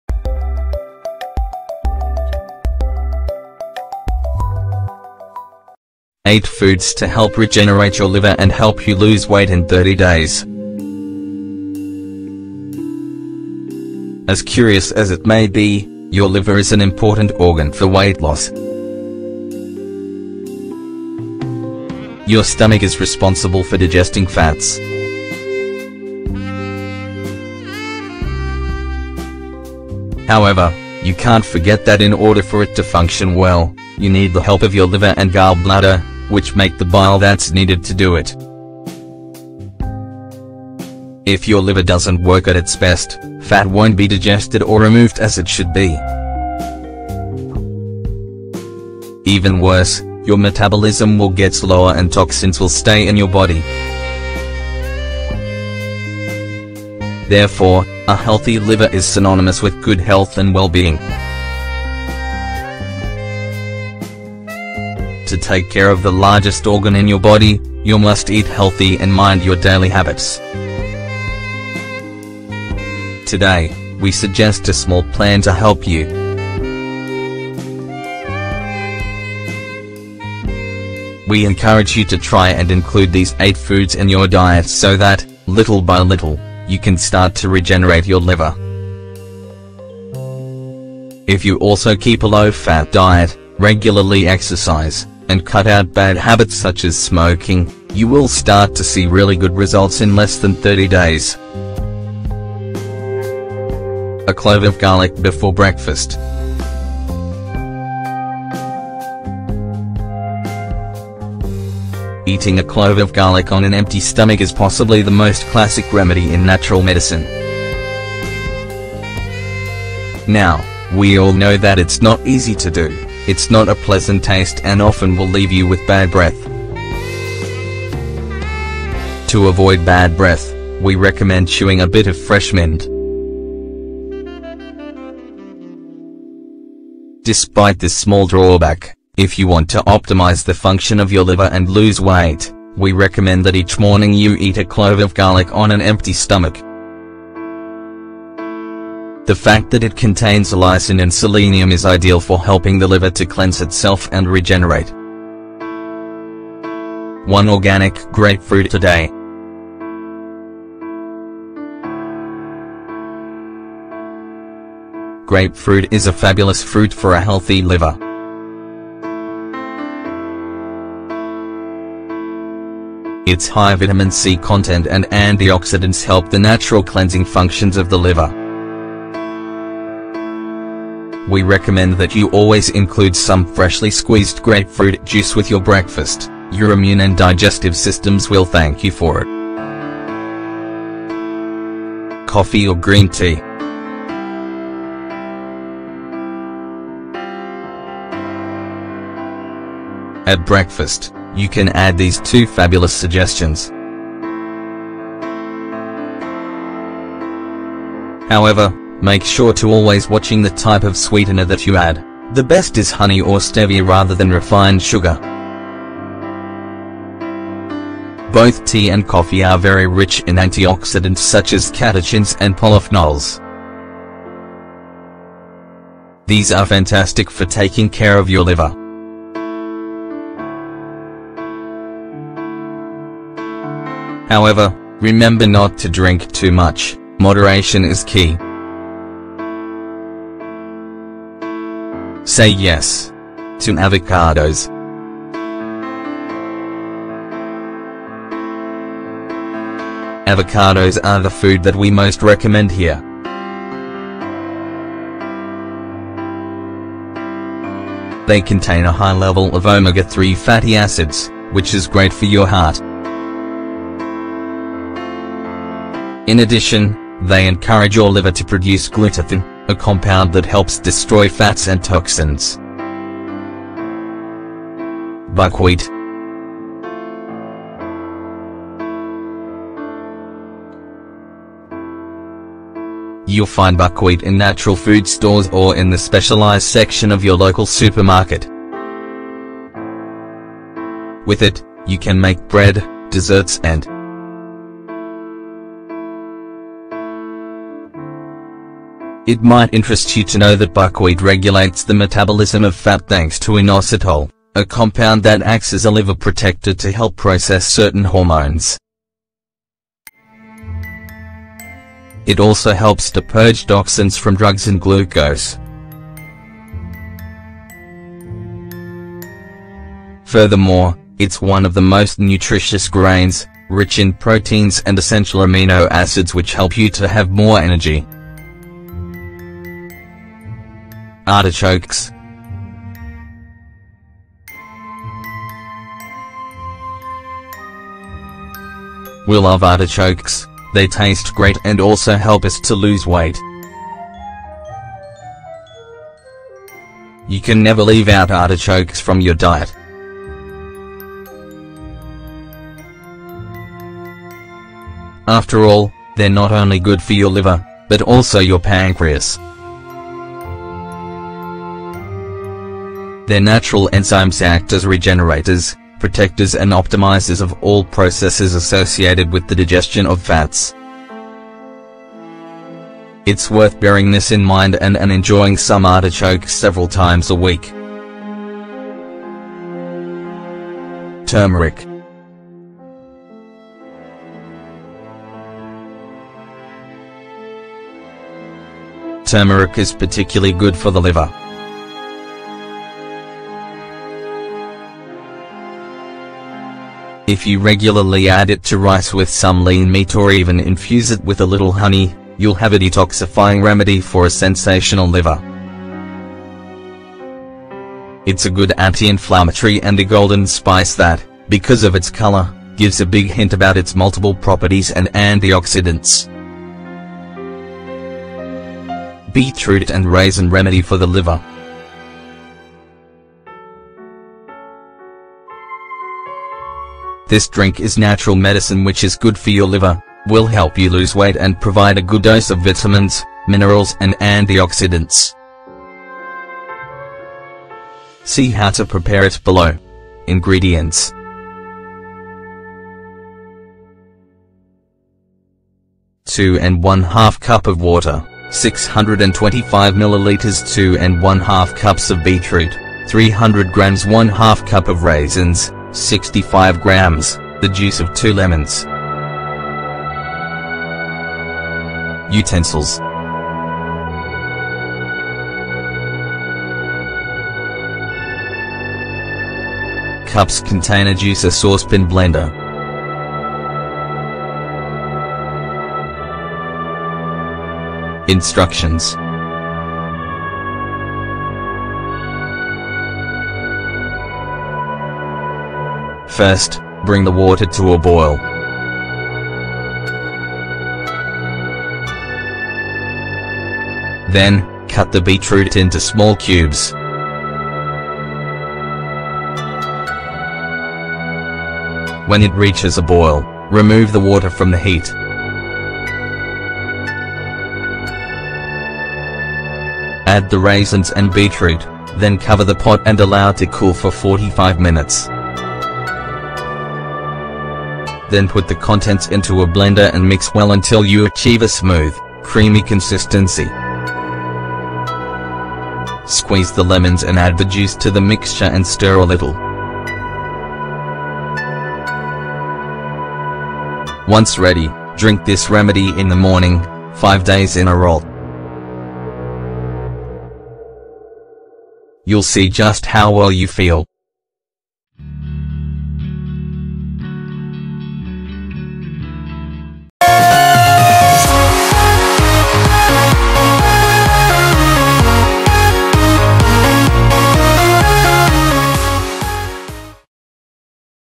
8 Foods to Help Regenerate Your Liver and Help You Lose Weight in 30 Days - Australia 360
To lose weight properly we have to care for and detoxify our liver regularly, since it’s responsible for digesting and removing excess fat
Healthy Life